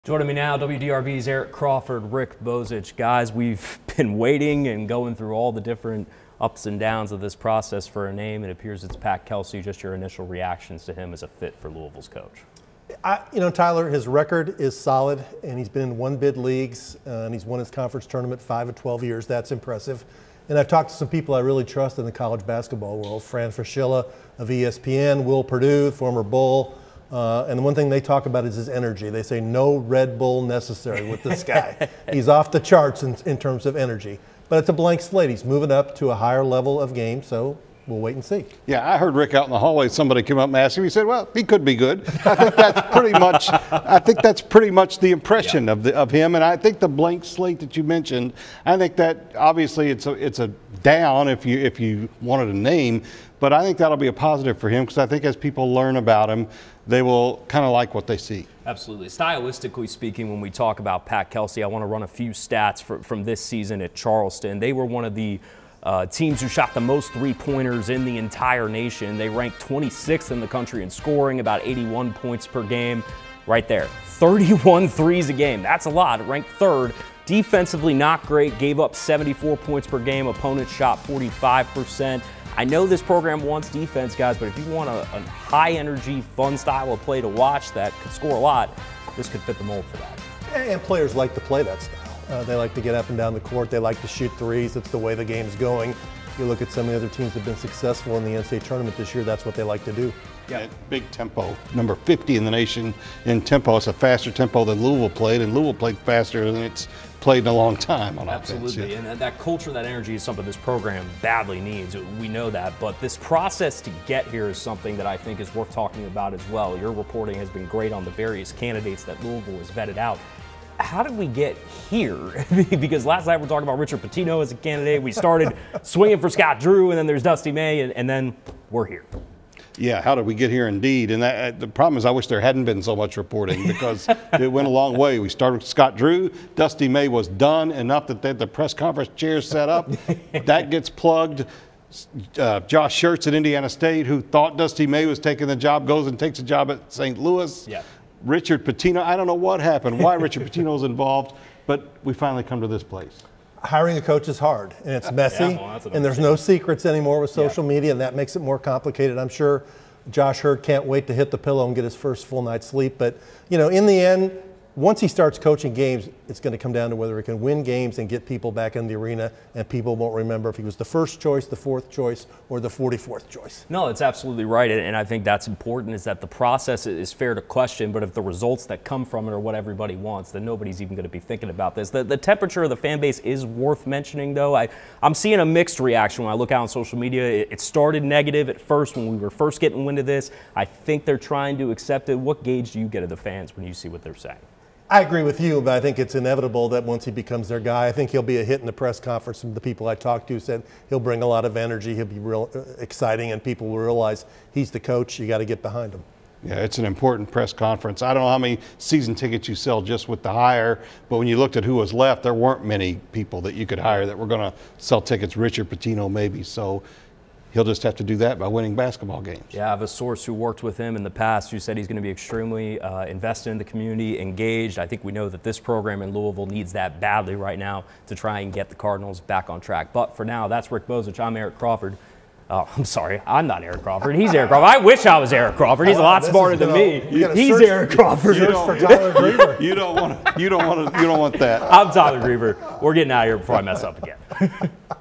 WDRB's Eric Crawford, Rick Bozich break down Louisville's hiring of Pat Kelsey from Charleston
The 48-year-old Cincinnati native was a standout at Elder High School there and played point guard at Xavier for Skip Prosser from 1995-98 after playing a season at Wyoming for former IU player Joby Wright.